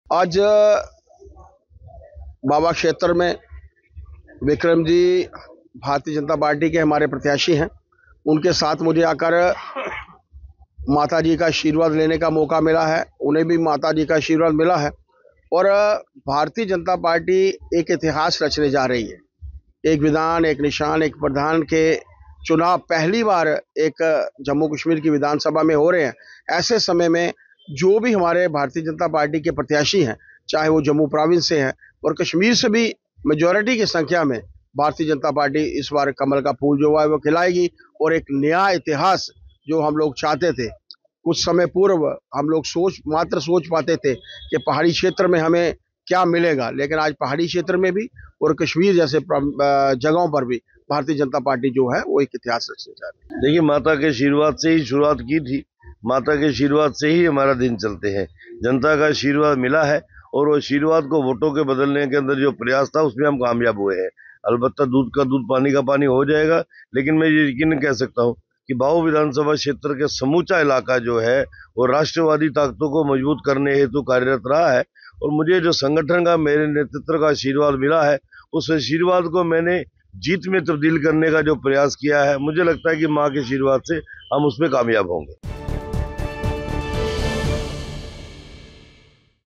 बीजेपी नेताओं ने जम्मू कश्मीर में जीत का किया दावा
जम्मू: आज जम्मू-कश्मीर की 90 विधानसभा सीटों के लिए मतगणना की जाएगी। मतगणना से पहले बीजेपी के प्रदेश कार्यकारी अध्यक्ष सत शर्मा ने कहा, विक्रम रंधावा हमारे बीजेपी प्रत्याशी हैं, और मुझे यहां आकर देवी मां का आशीर्वाद लेने का सौभाग्य मिला है। उन्हें उनका आशीर्वाद भी मिला है। बीजेपी आगामी चुनावों में इतिहास बनाने वाली है।वहीं बाहु विधानसभा क्षेत्र से बीजेपी प्रत्याशी विक्रम रंधावा ने कहा, "हमने अपने दिन की शुरुआत देवी मां के आशीर्वाद से की है और उन्हीं के आशीर्वाद से हम आगे बढ़ते हैं। हमें जनता का समर्थन मिला है और हम उन आशीर्वादों को वोटों में बदलने में सफल हुए हैं।"